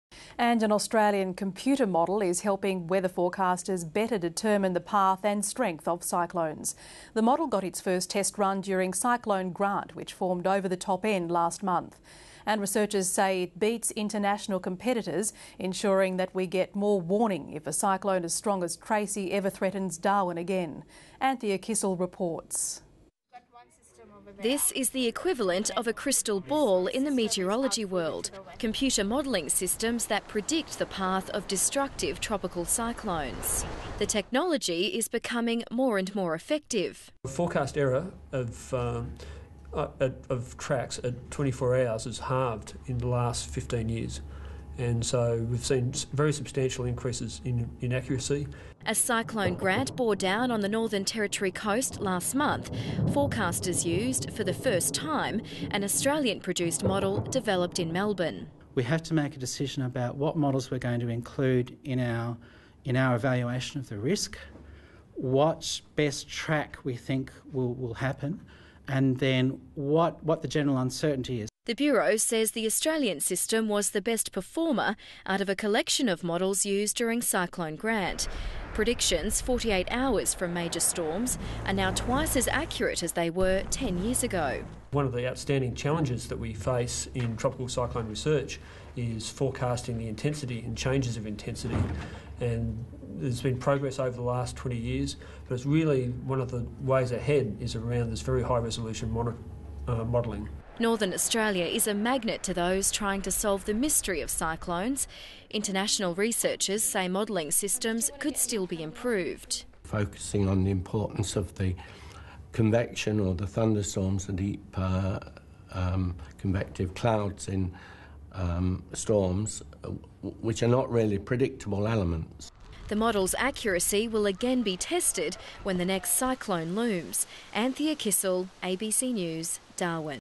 Scientists use new ways of predicting cyclones
The Bureau of Meteorology successfully trialled high resolution weather monitoring during Cyclone Grant when it hit the Northern Territory in late 2011.